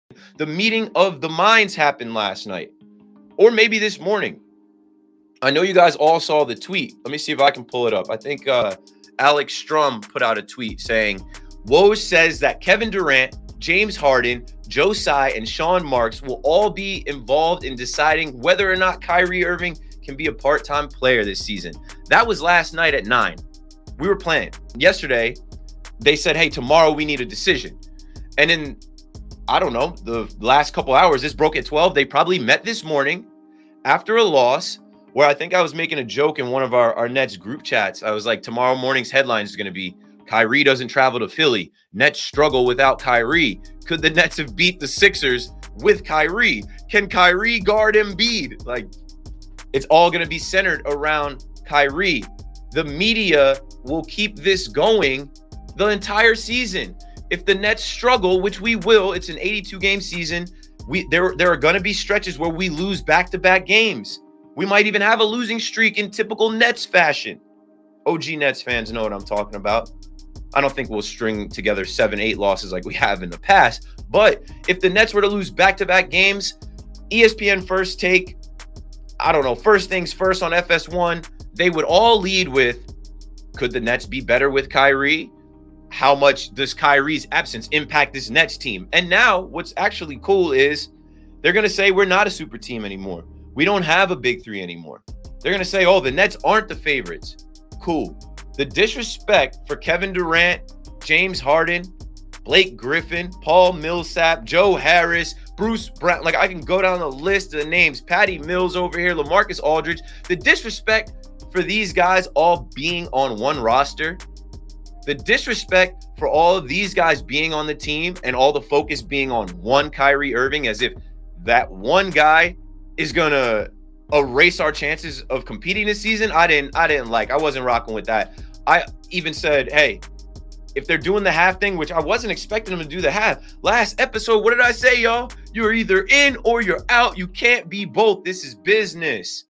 One week til tip off! Nets move on without Kyrie..
The meeting of the minds happened this morning. A decision was made to move forward without Kyrie. People celebrated this like the Nets' season is over? This was the RIGHT move. Now lock in. The disrespect of the rest of the roster should be even more MOTIVATION! Still Nets in 4.

Call and leave a voicemail: (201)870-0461